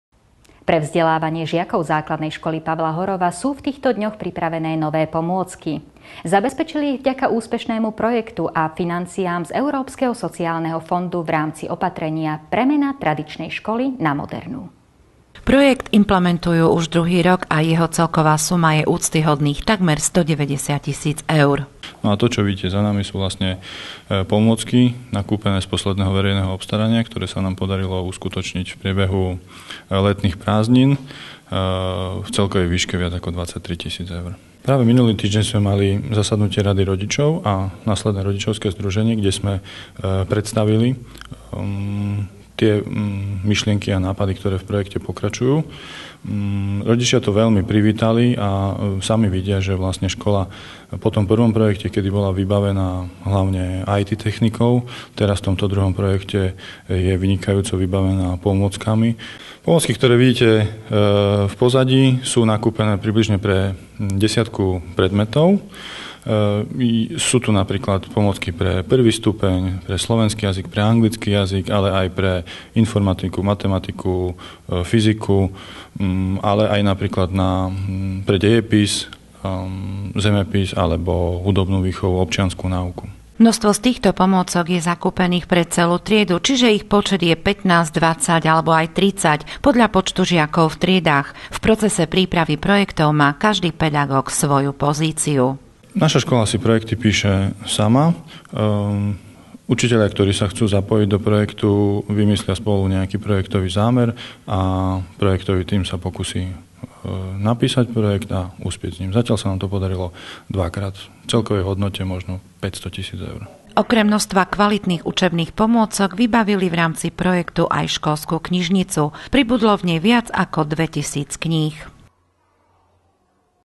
tvmistral modernaskola
Dna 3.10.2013 odvysielala TV Mistral prispevok o modernom vzdelavani v nasej skole. Prispevok obsahuje informacie o novych pomockach nakupenych v ramci projektu z ESF.